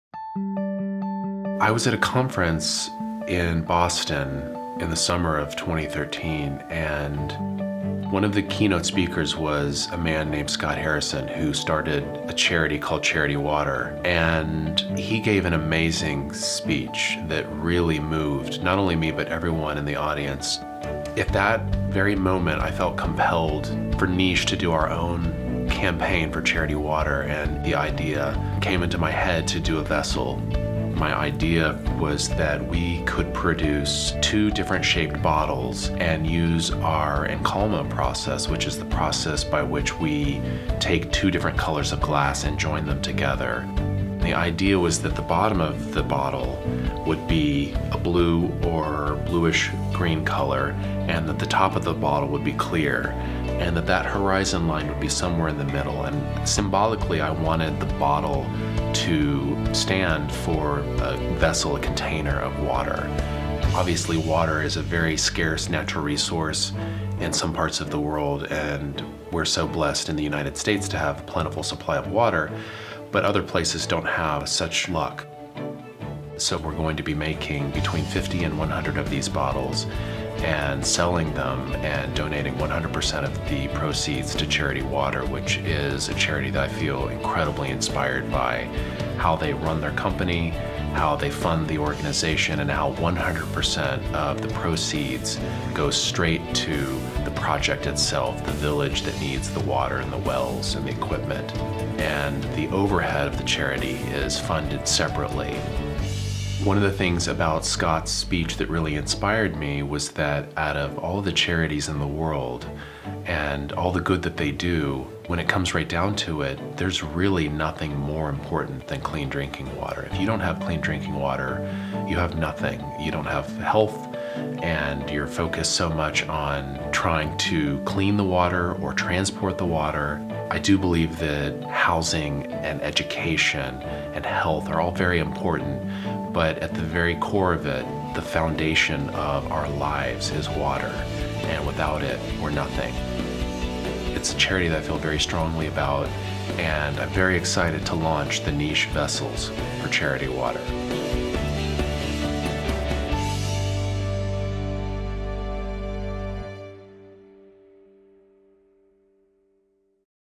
Niche Handmade Glass Products - Niche x Charity Water
The Niche Modern H2O series has been custom designed for our collaboration with charity: water. Each bottle is handmade using te encalmo technique and joinging together two separate forms into one finished vessel. This is perfectly symbolic of our endeavor to join together with our customers to realize clean drinking water solutions for those in dire need. Each H20 series is completely handmade and unique; available in Sapphire + Clear or Condesa + Clear glass. 100% of proceeds will be donated to charity: water.